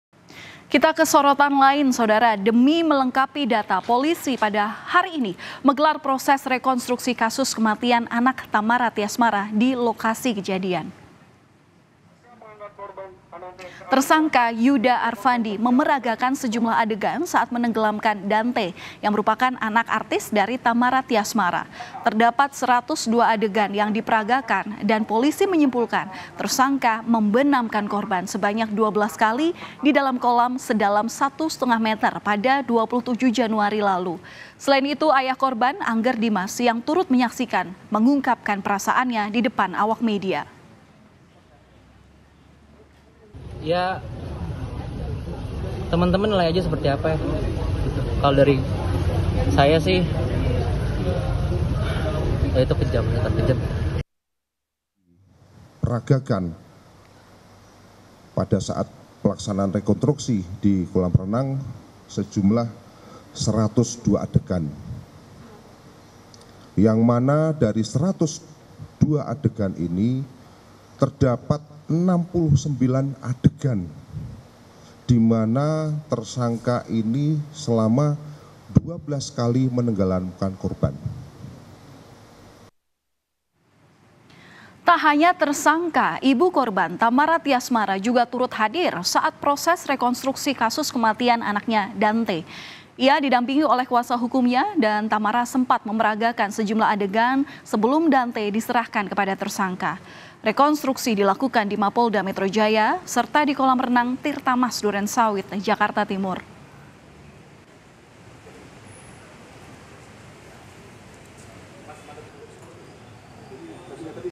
Kata Ayah Kandung Dante usai Saksikan Rekonstruksi Pembunuhan
JAKARTA, KOMPAS.TV - Demi melengkapi data, polisi hari ini menggelar proses rekonstruksi kasus kematian anak Tamara Tyasmara di lokasi kejadian.

Tersangka Yudha Arfandi memeragakan sejumlah adegan saat menenggelamkan Dante, Anak Artis Tamara Tyasmara.

Terdapat 102 adegan yang diperagakan dan polisi menyimpulkan tersangka membenamkan korban sebanyak 12 kali di dalam kolam sedalam 1,5 meter pada 27 Januari 2024.

Selain itu Ayah Korban, Angger Dimas yang turut menyaksikan, mengungkapkan perasaannya di depan awak media.

Kompas TV
Independen | Terpercaya